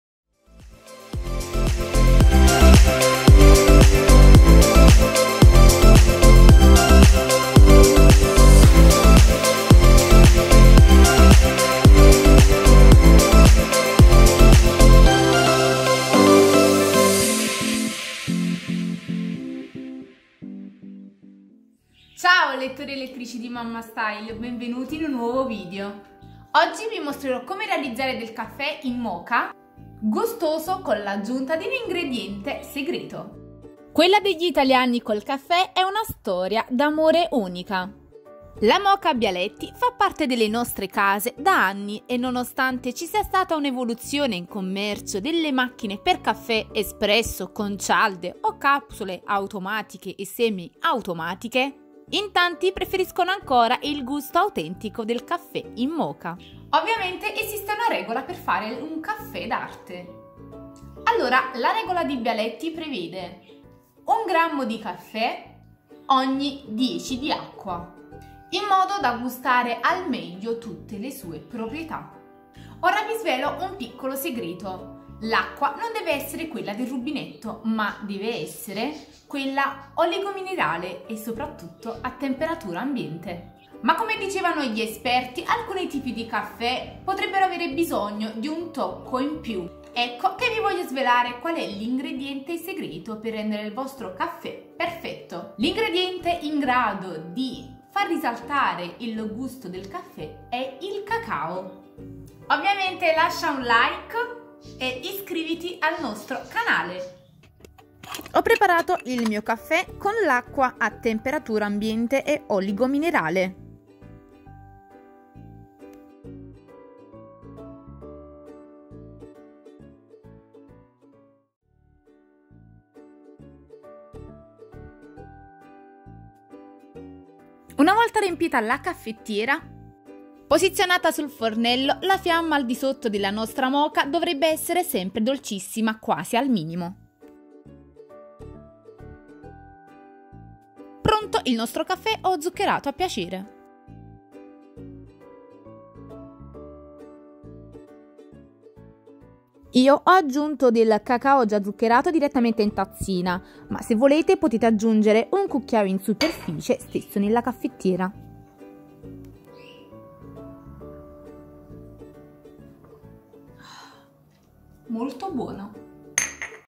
Il tuo caffè non ha sapore? Aggiungi questo ingrediente nella MOKA e il risultato sarà SORPRENDENTE!
Se il caffè della tua moka è poco gustoso, prova ad aggiungere questo ingrediente, non avresti mai immaginato lo straordinario sapore!

LE NOSTRE CATEGORIE